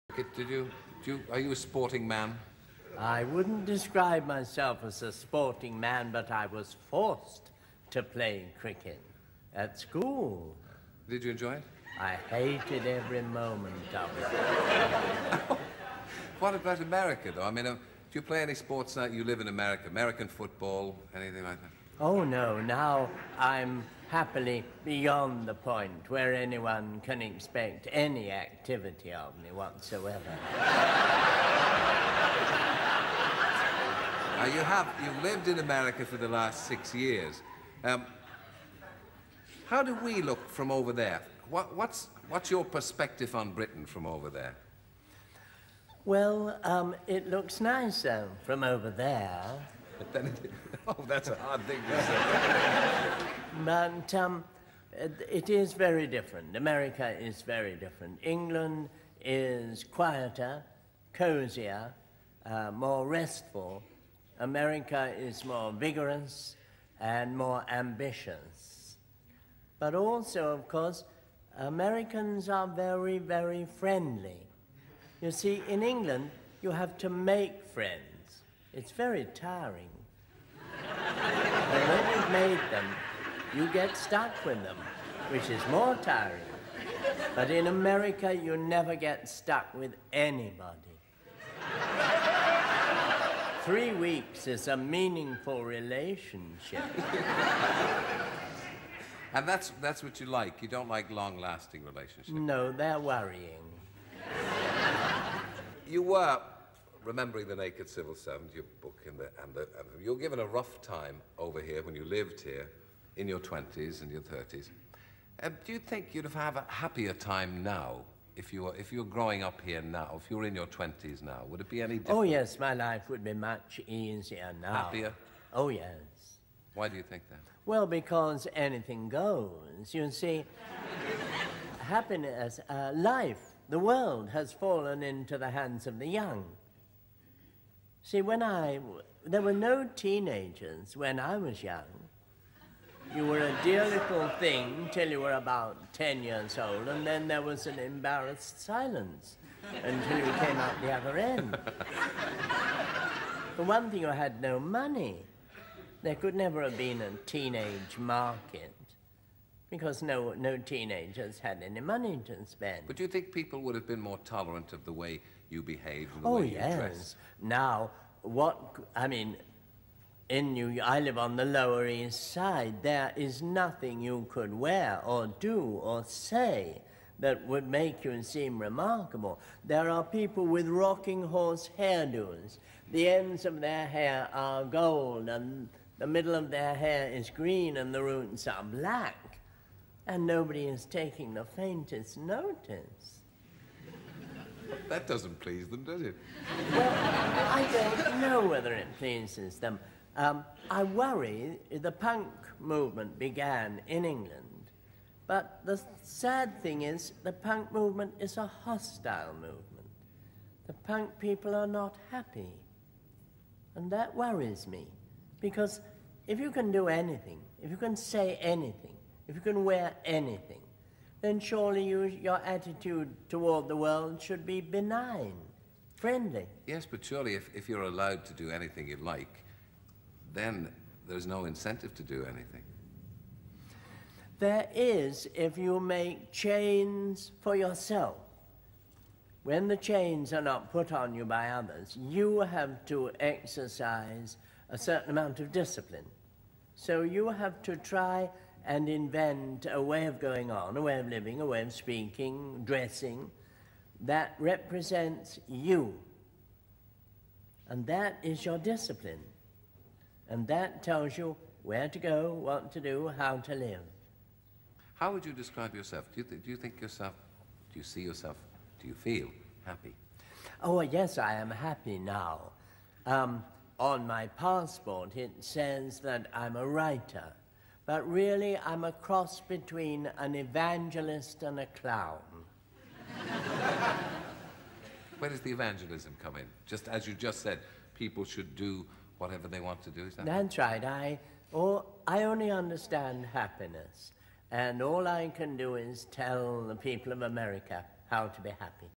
Quentin Crisp (1908-1999) writer, raconteur and actor
Quentin Crisp (born Denis Charles Pratt; 25th December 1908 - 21st November 1999) was an English writer, raconteur and actor. in conversation with Terry Wogan (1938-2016) in 1987.